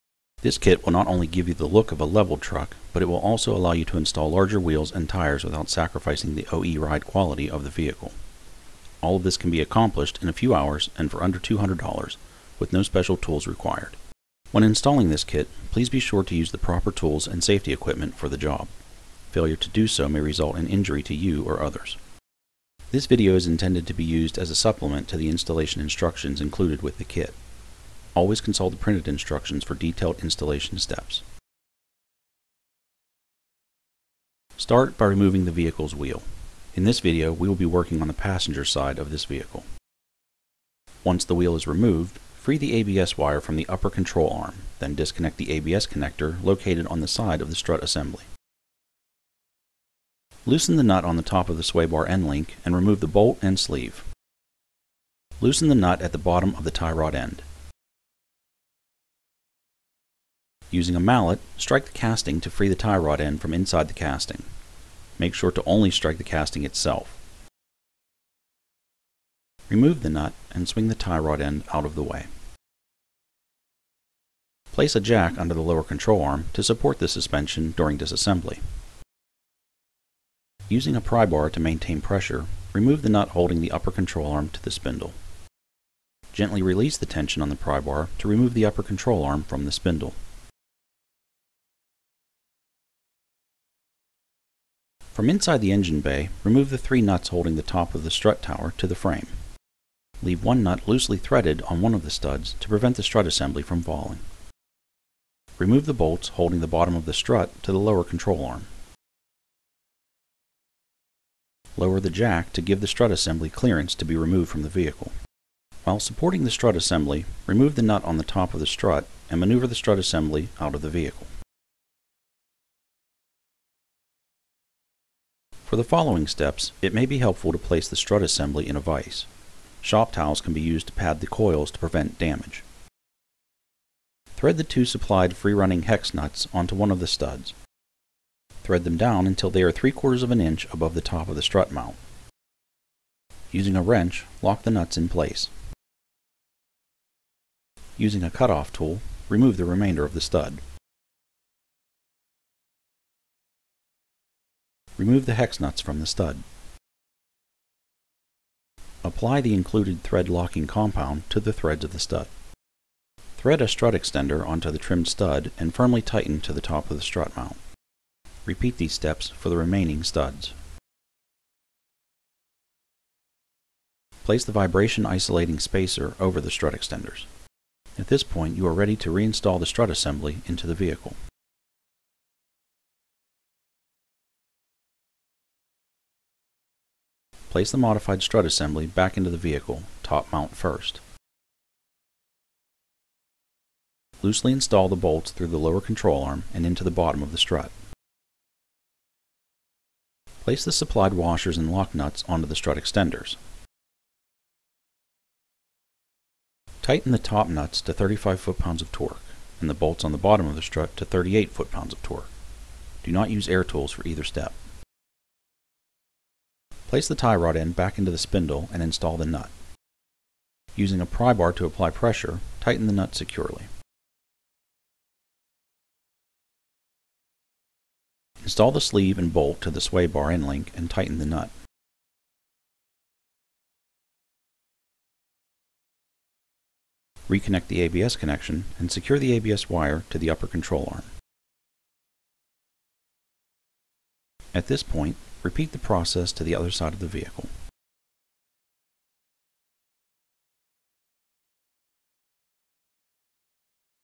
How to Install Ground Force Suspension Leveling Kit
The Ground Force leveling Kit lifts the front of your truck for a level stance. Subjected to rigorous testing to give you the best quality product.

VIdeo Transcription:
This kit will not only give you the look of a level truck, but it will also allow you to install larger wheels and tires without sacrificing the OE ride quality of the vehicle. All his can be accomplished in a few hours and for under $200 with no special tool required.
When installing this kit, please be sure to use the proper tools and safety equipment for the job. Failure to do so may result in injury to you or others. This video is intended to be used as a supplement to the installation instructions included with the kit. Always consult the printed instructions for detailed installations steps.
Start by removing the vehicle's wheel. In this video, we'll be working on the passenger's side of this vehicle. Once the wheel is removed, free the AVS wire from the upper control arm, then disconnect the AVS connector located on the side of the strut assembly. Loosen the knot on the top of the sway bar end link and remove the bolt and sleeve. Loosen the knot at the bottom of the tie rod end. Using a mallet, strike the casting to free the tie rod end from inside the casting, make sure to only strike the casting itself. Remove the nut and swing the tie rod end out of the way. Place a jack under the lower control arm to support the suspension during dis-assembly. Using a pry bar to maintain pressure, remove the nut holding the upper control arm to the spindle. Gently release the tension on the pry bar to remove the upper control arm from the spindle.
From inside the engine bay, remove the three nuts holding the top of the strut tower to the frame. Leave one nut loosely threaded on one of the studs to prevent the strut assembly from falling. Remove the bolts holding the bottom of the strut to the lower control arm. Lower the jack to give the strut assembly clearance to be removed from the vehicle. While supporting the strut assembly, remove the nut on the top of the strut, and maneuver the strut assembly out of the vehicle.
For the following steps, it may be helpful to place the strut assembly in a vice. Shop towels can be used to pad the coils to prevent damage. Thread the two supplied free-running hex nuts onto one of the studs. Thread them down until they're three quarters of an inch above the top of the strut mount. Using a wrench, lock the nuts in place. Using a cut-off tool, remove the remainder of the stud. Remove the hex nuts from the stud.